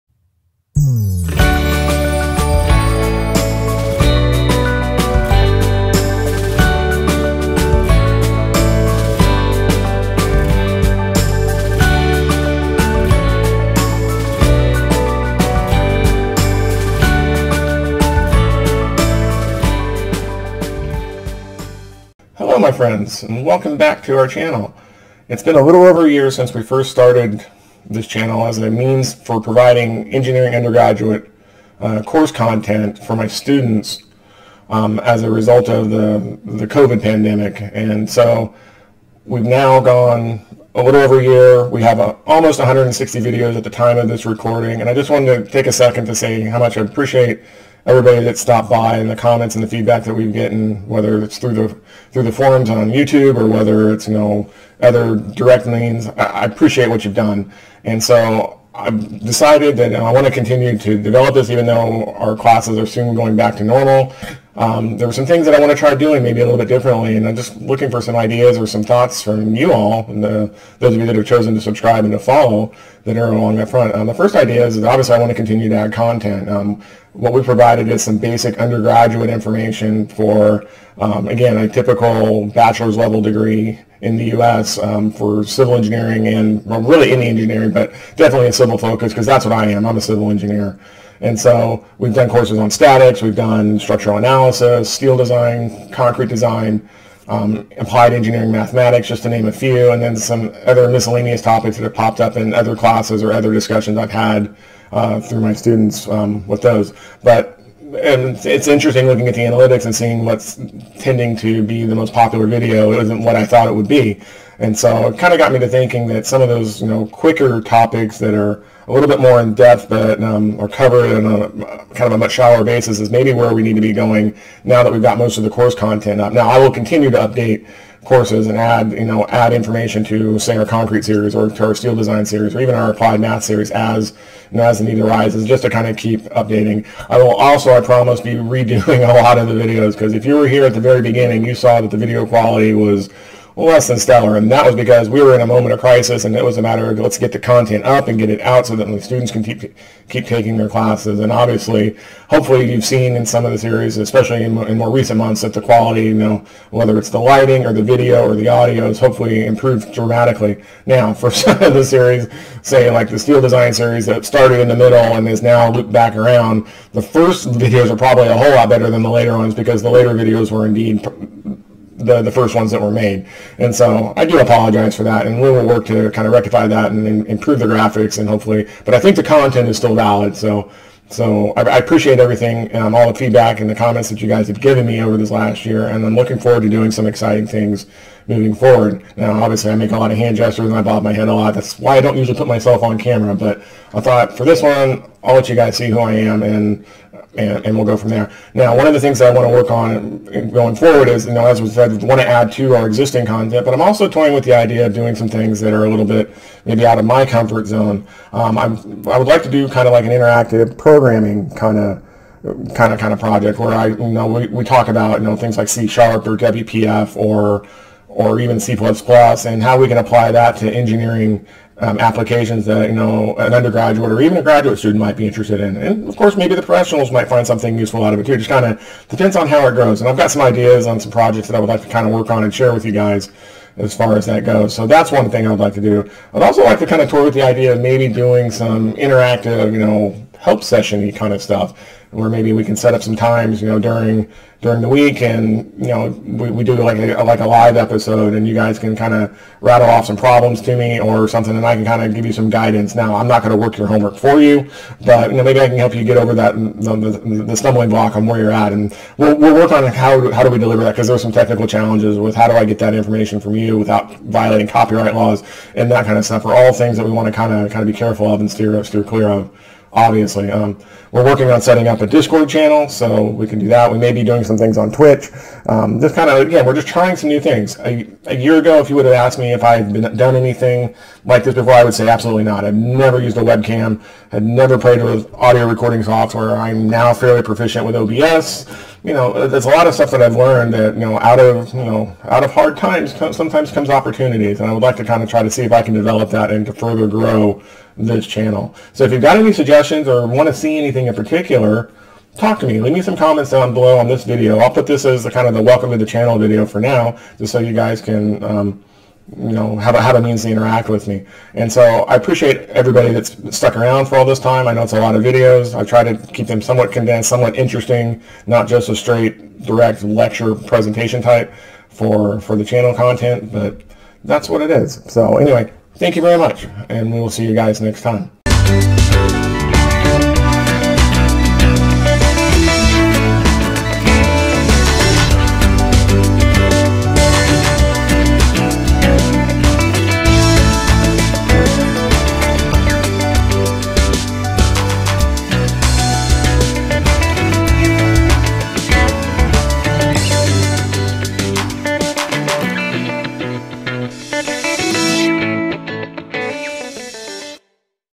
JayThree Engineering Courses - One Year Later
Thanks to the subscribers who have joined over the past 13 months.  Our channel has evolved and we are happy to introduce some exciting new projects coming to the channel in the near future.

We are also asking for your input as to what new content you might be interested in seeing. So leave us some comments down below, and as always, thanks for supporting us.  Happy Engineering!